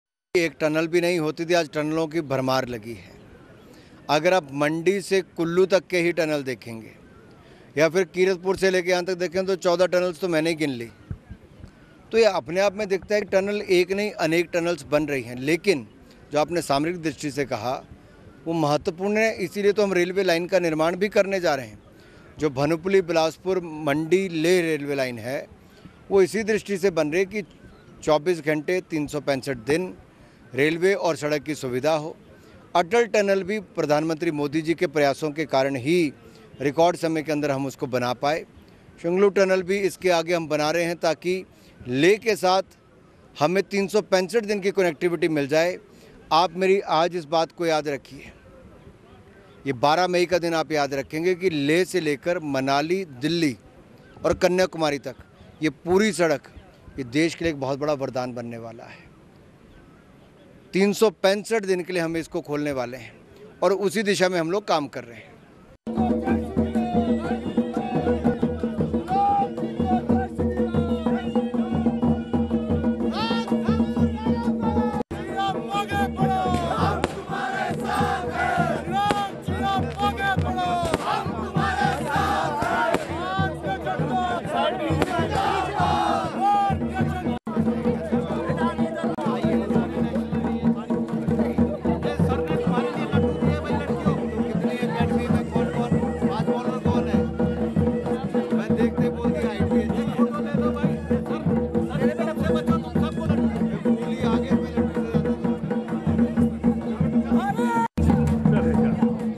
Kullu पहुंचे केंद्रीय मंत्री Anurag Thakur ने क्यों कहा ‘12 मई की तारीख रखना’ । TV9HimachalPradesh
लोकसभा चुनाव के मद्देनजर हिमाचल प्रदेश में अपनी स्थिति मजबूत करने के लिए केंद्रीय मंत्री अनुराग ठाकुर हिमाचल के अलग-अलग जिलों का दौरा कर रहे हैं. इस कड़ी में वो पहुंचे कुल्लू जहां उन्होंने कहा कि सामरिक दृष्टिकोण के लिए रेलवे का प्रोजेक्ट बहुत महत्वपूर्ण है, साथ ही उन्होेंने कहा कि राज्य में बहुत सी टनल बनी हैं आगे भी बनेंगी.